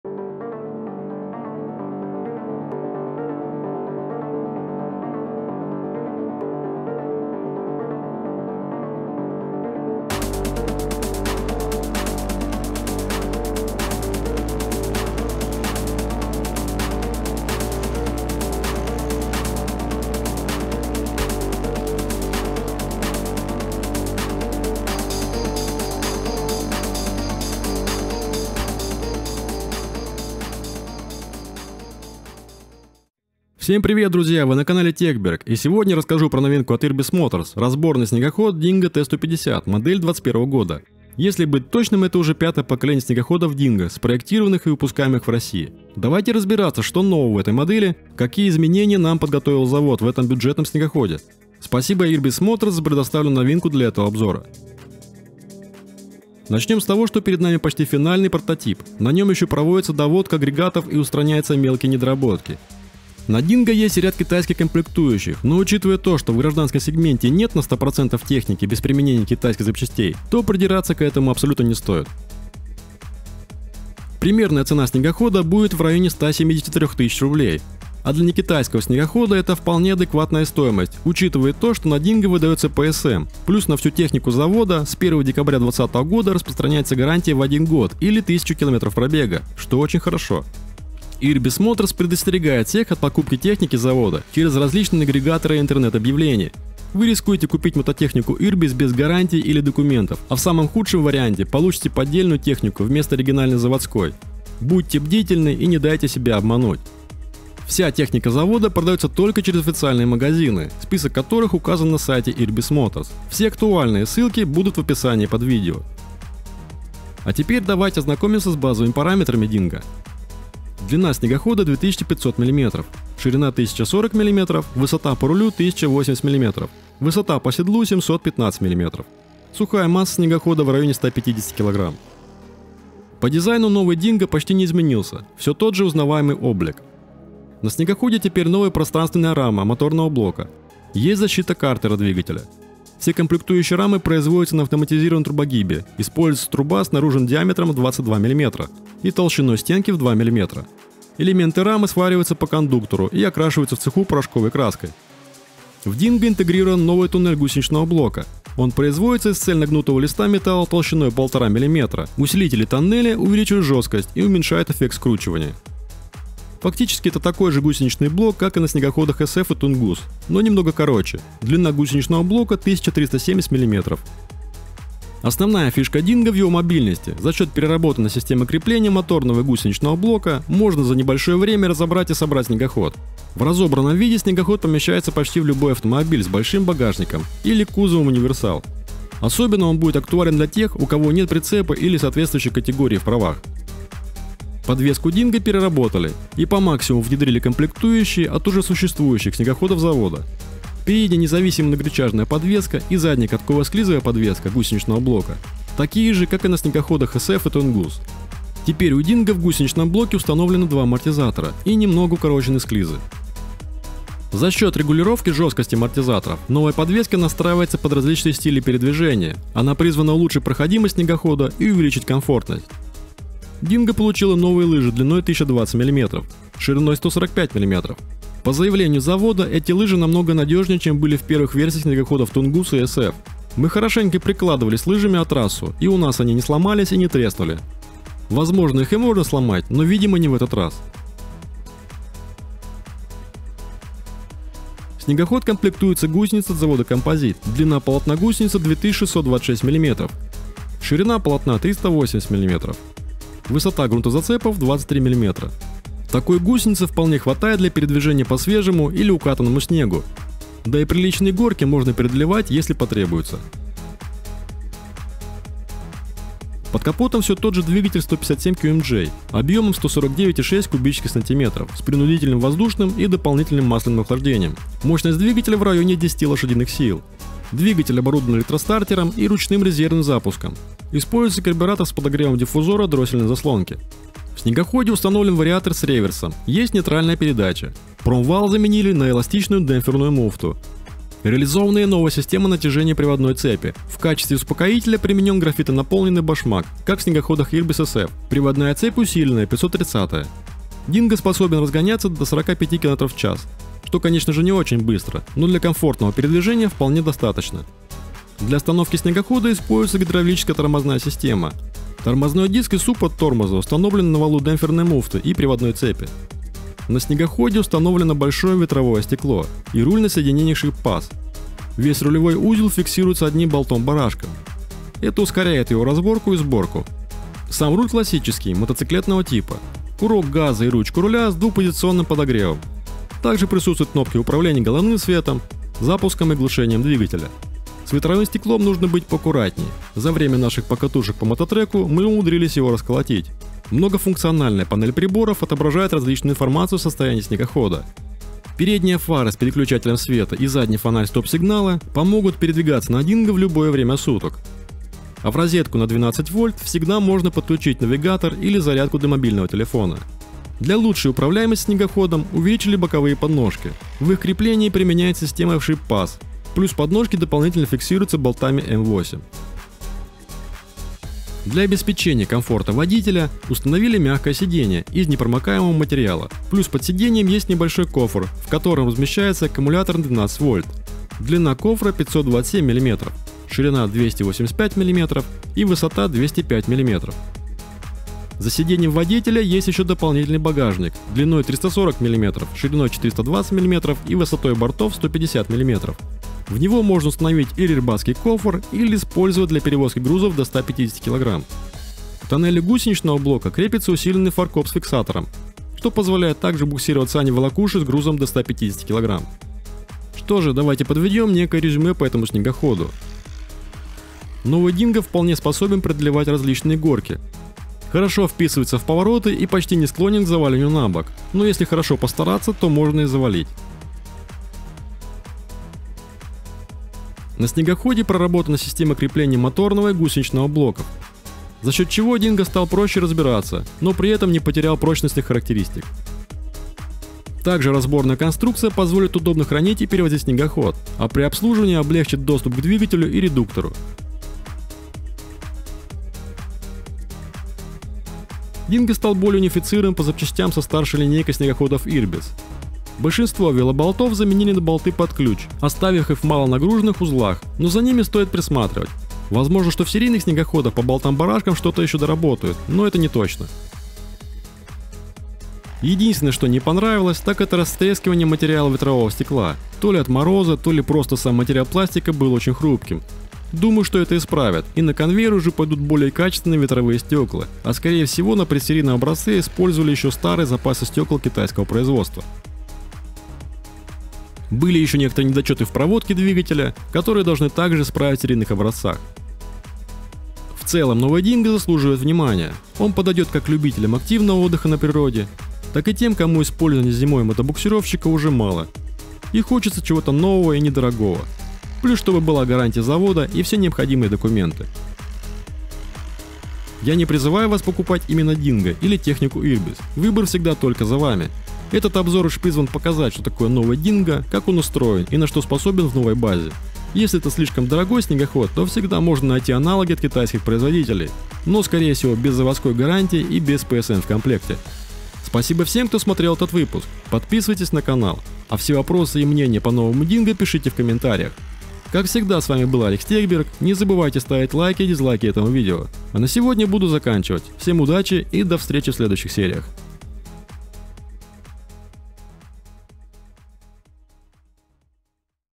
Снегоход Ирбис Динго Т150. Самый бюджетный Российский снегоход | обзор снегохода Dingo T150
Самый бюджетный снегоход Dingo T150 от Ирбис Моторз, все технические подробности снегохода Динго Т150 смотрите в этом видео обзоре. Спасибо Ирбис Моторз за предоставление снегохода для обзора. Снегоход ожидается в продаже в конце января 2021г, актуальную цену на снегоход Динго и всю технику завода смотрите на официальном сайте Ирбис Моторо.

Оставляйте комментарии, это помогает видео быть в рекомендациях.